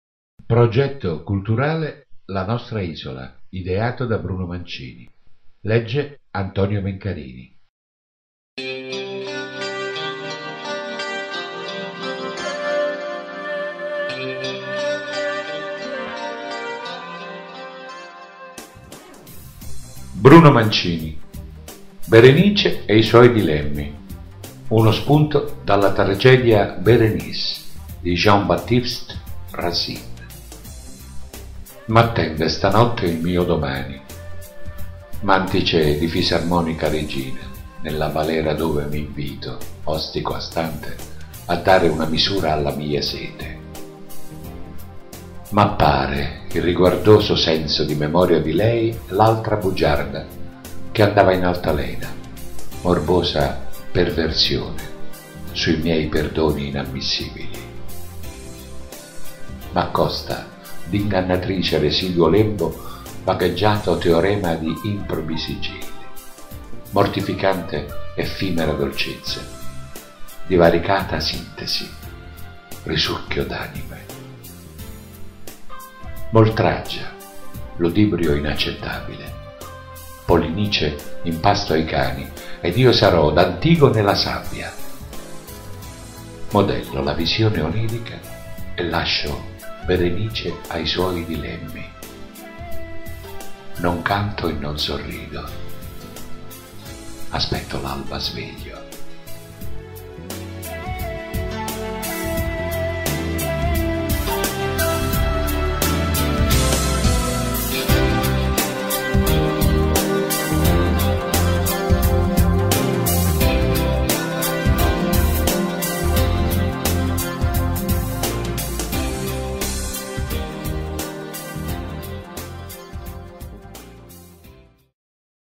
Berenice e i suoi dilemmi Antonio Mencarini video cantante
Dalla raccolta di poesie “Sasquatch”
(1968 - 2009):
Berenice e i suoi dilemmi

M’attende
stanotte il mio domani,
mantice di fisarmonica regina
nella balera dove m’invito,
ostico astante.
a dare una misura alla mia sete.

M’appare
irriguardoso senso di memoria
di lei, l’altra bugiarda,
che andava in altalena,
morbosa perversione,
sui miei perdoni inammissibili.

M’accosta
d’ingannatrice residuo lembo
vagheggiato teorema di improbi sigilli
mortificante effimera dolcezza
divaricata sintesi
risucchio d’anime.

M’oltraggia
ludibrio inaccettabile,
Polinice in pasto ai cani
ed io sarò d’Antigone la sabbia.
Modello la visione onirica
e lascio Berenice ai suoi dilemmi.

Non canto e non sorrido.

Aspetto l’alba sveglio.

Uno spunto dalla tragedia “Berenice” di Jean-Baptiste Racine (La Ferté-Milon, 22 /12/1639 – Parigi, 21/04/1699)